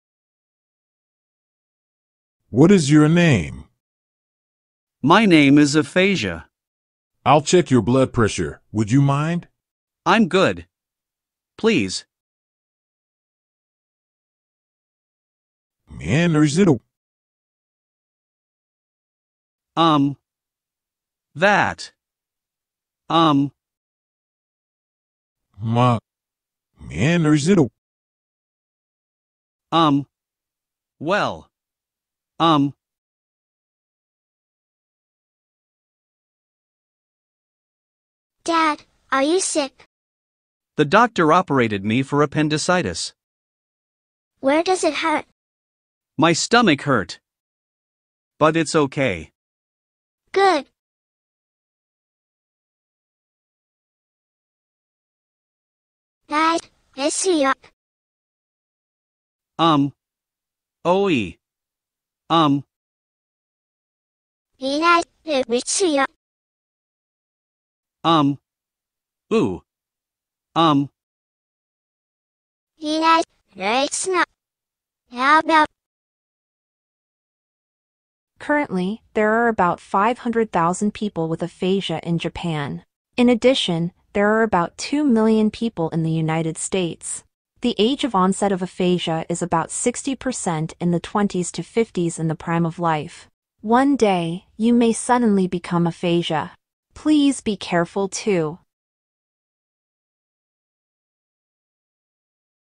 I am aphasia. I can't communicate verbally.
On November 27, 2019, I had a stroke in my left brain.
Of course, I became aphasic. I don't understand the language.
It's like this. Please take a look.